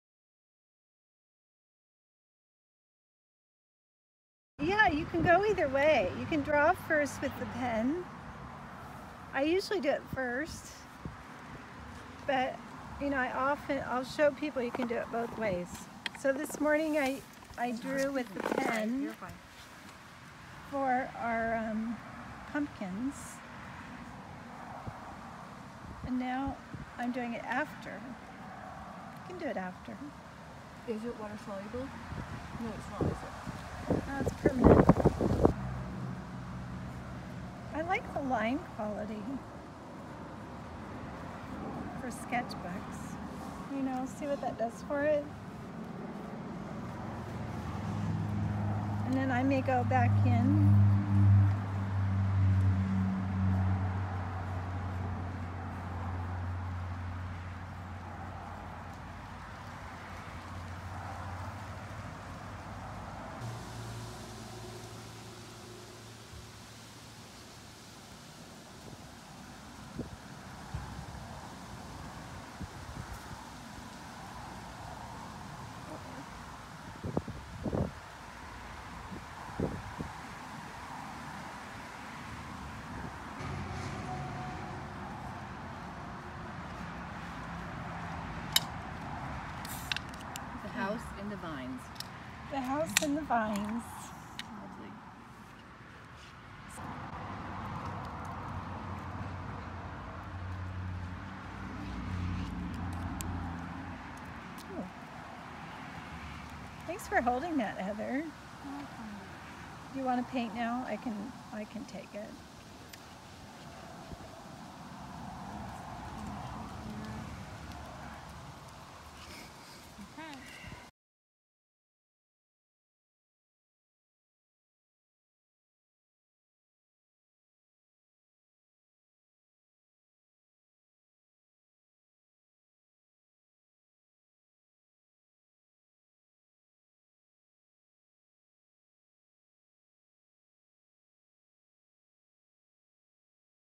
Adding Linework to Watercolors: Art Technique by Cindy Briggs
Cindy Briggs shares how she adds linework to her colorful sketchbook watercolors.  This plein air demonstration was filmed at the Carmel Retreat, October 2018.